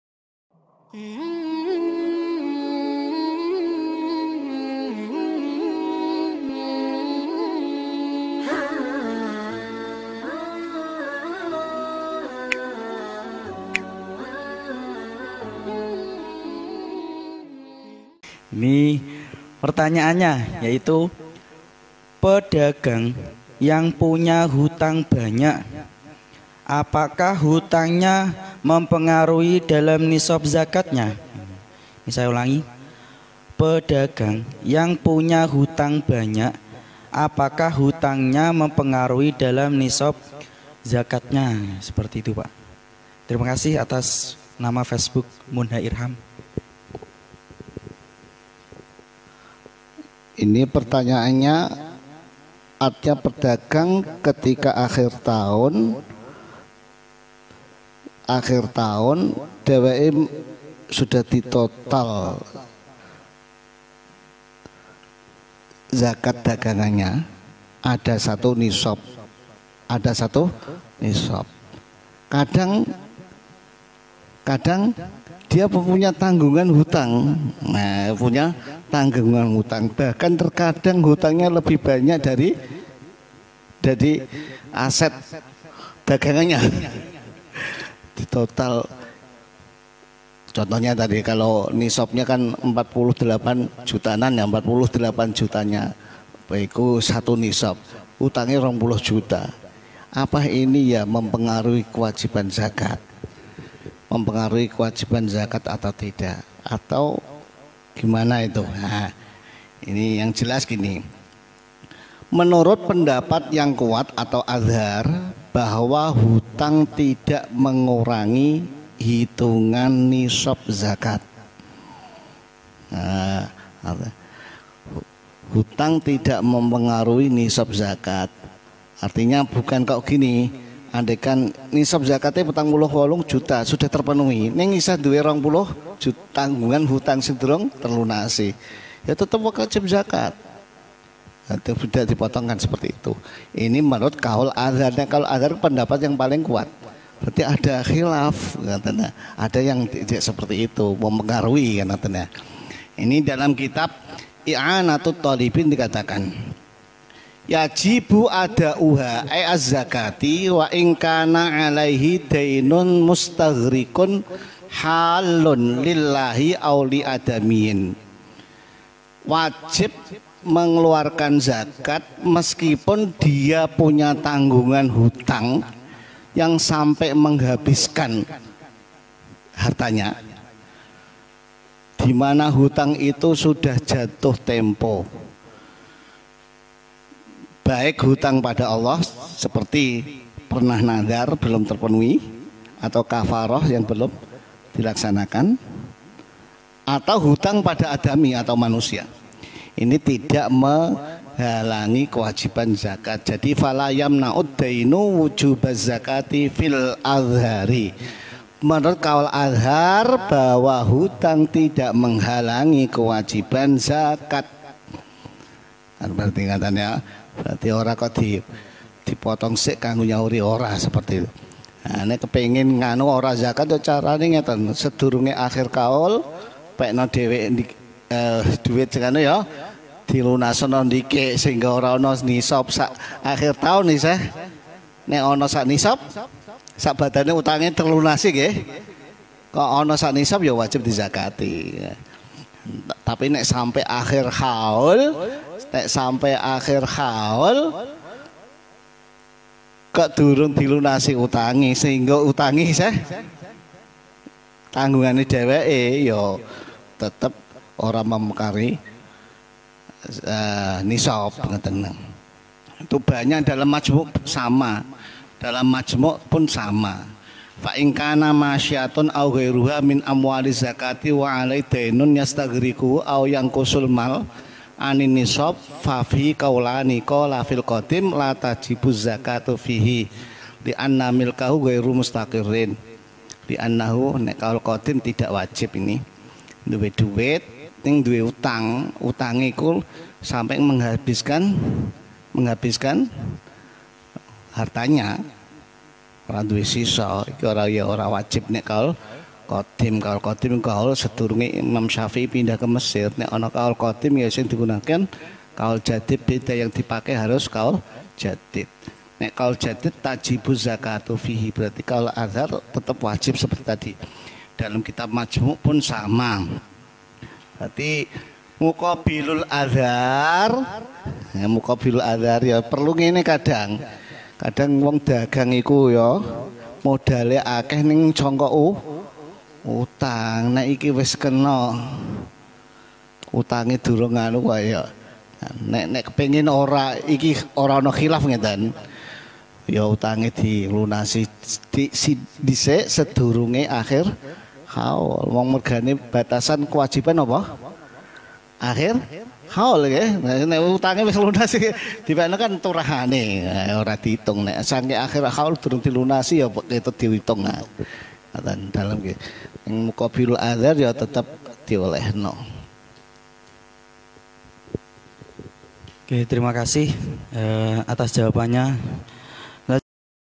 KH Subhan, Pedagang Punya Hutang, Apakah Mempengaruhi Nishab Zakat
KH Subhan merupakan seorang ulama' muda dari kudus, beliau dikenal sebagai salah satu Aktivis LBMNU Kudus, dalam sebuah kesempatan tanya jawab seputar zakat disalah satu masjid di daerah Ngemplak Undaan Kudus beliau menjelaskan tentang Pedagang Punya Hutang, Apakah Mempengaruhi Nishab Zakat
Ngaji Zakat bareng Yai Subhan Kudus